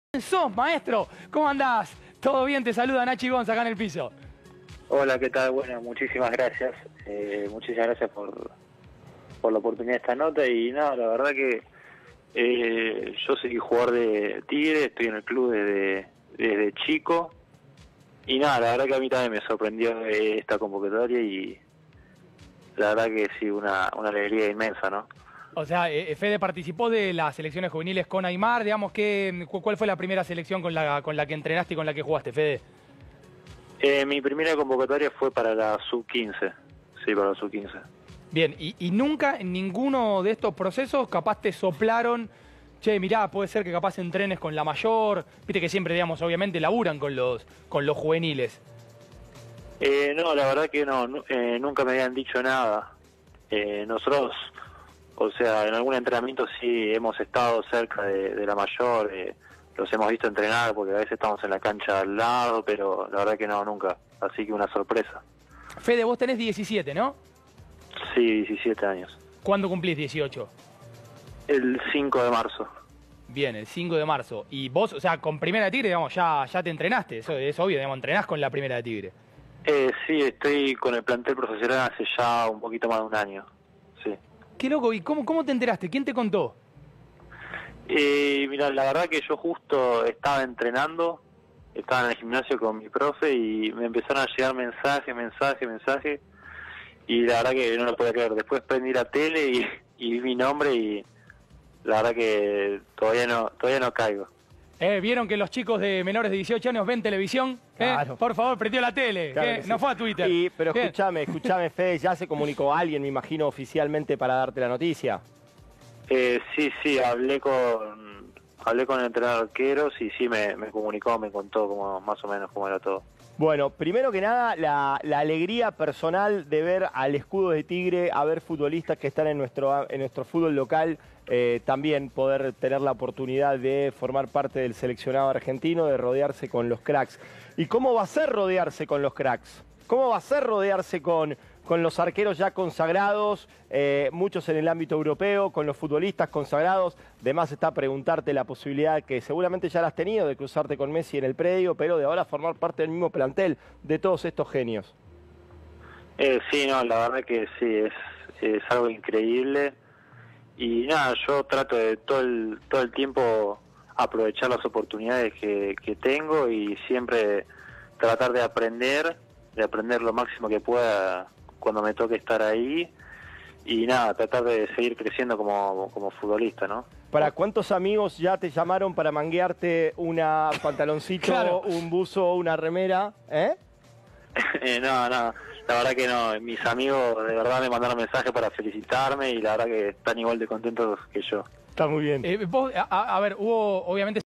FEDERICO GOMES GERTH, MANO A MANO CON TNT SPORTS
"TRATARÉ DE APRENDER LO MÁXIMO QUE PUEDA ESTANDO AHÍ"
Federico Gomes Gerth, arquero de Tigre convocado a la Selección Argentina 👏🇦🇷